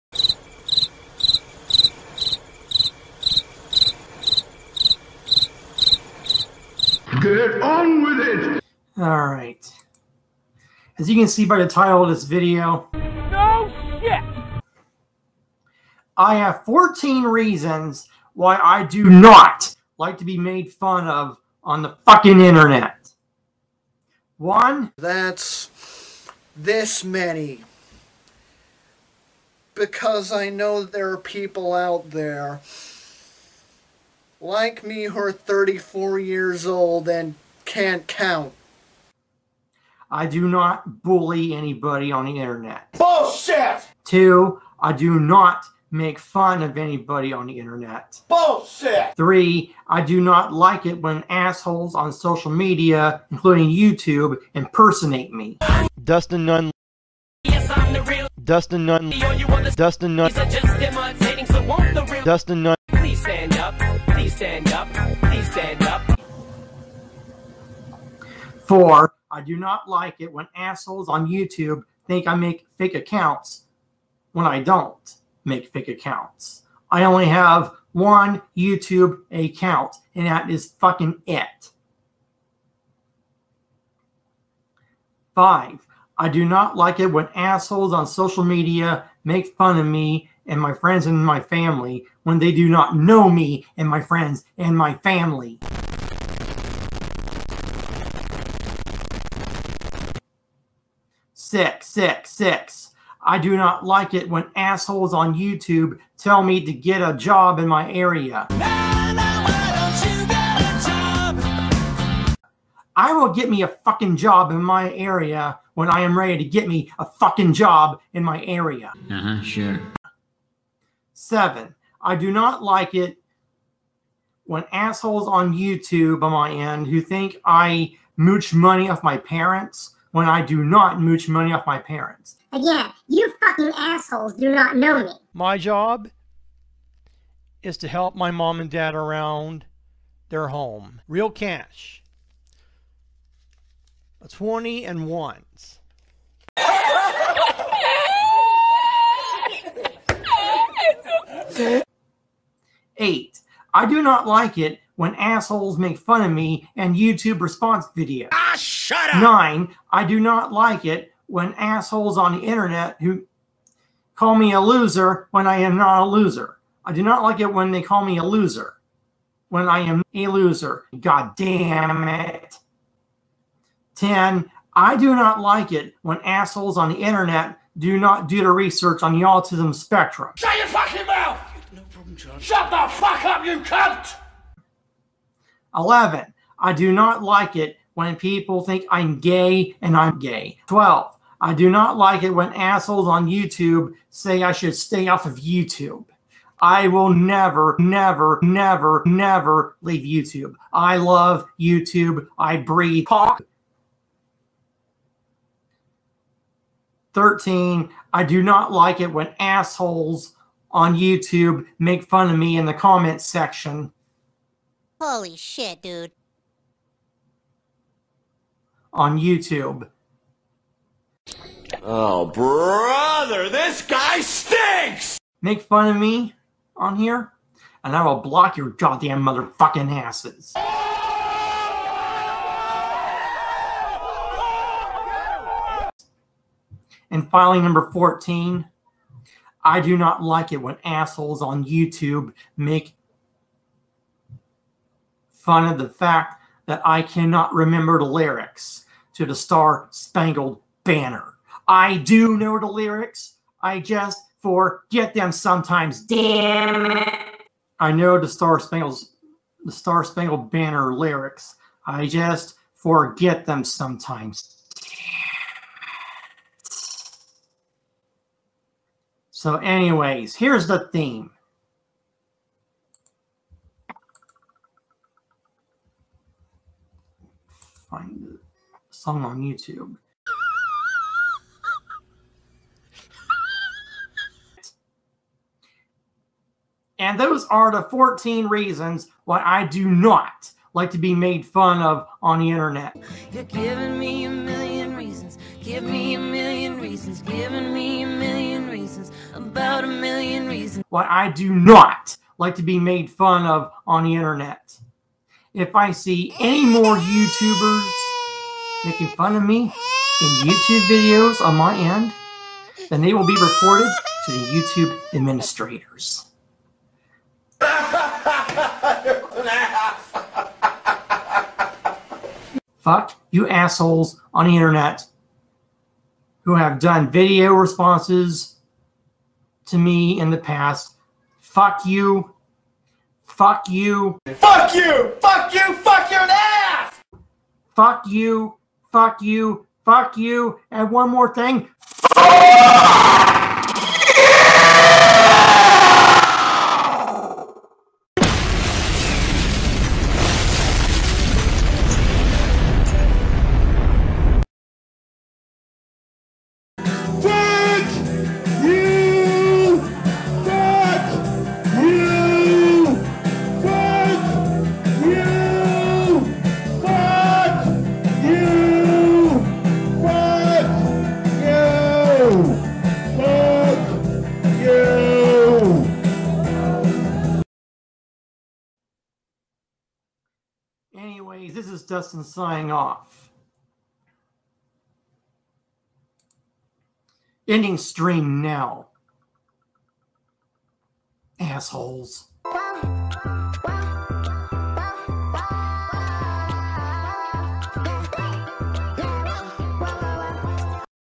YTP: Dustin Nunn Does the F You Challenge to Own His Haters
Dustin Nunn does this legendary challenge, and only few have done it to this level, especially when owning their detractors.
Shoutout to @TheSkyatollah for giving me material to make this vid even more hilarious.  He is shown at 0:25 parodying Dustin Nunn